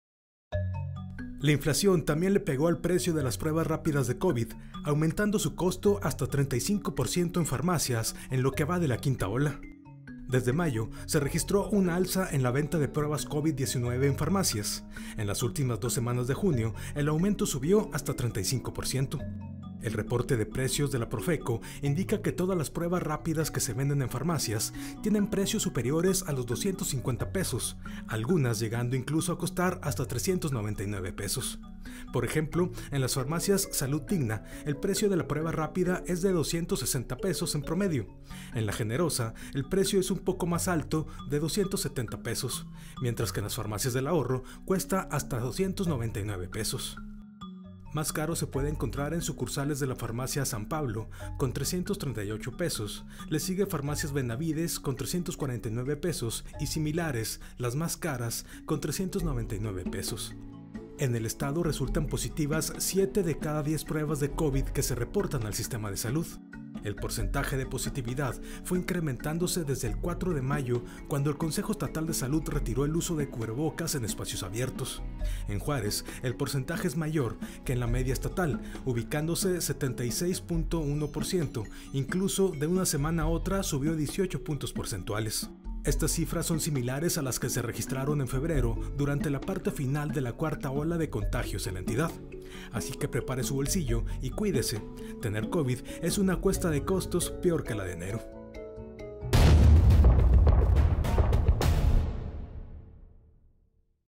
Las pruebas caras de Covid-19
La inflación también le pegó al precio de las pruebas rápidas de Covid-19, aumentando su costo hasta 35 por ciento en farmacias en lo que va de la quinta ola de contagios.

Un reporte de precios de la Profeco indica que todas las pruebas rápidas que se venden en farmacias tienen precios superiores a los 250 pesos; algunas llegando incluso a costar hasta 399 pesos.